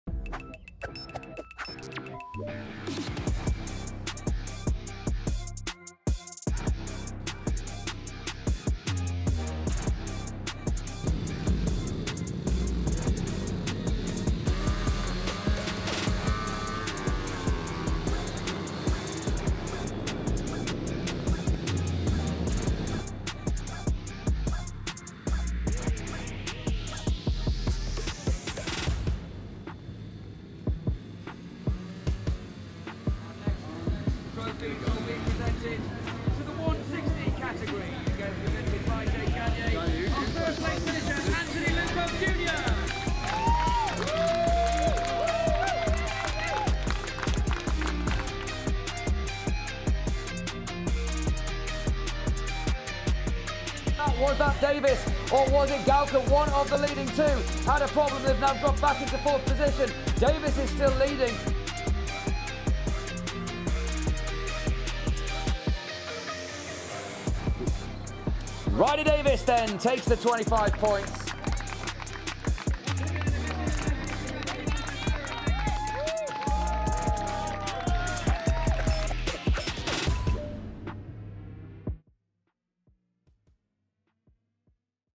MotoAmerica Mini Cup by Motul Championship: 160 Highlights at Pittsburgh 2022
The sweet spot in the Mini Cup by Motul USA Championship is the 160cc class, which feeds riders to the FIM Mini GP Cup. Here are the highlights from both 160cc races at the Pittsburgh round.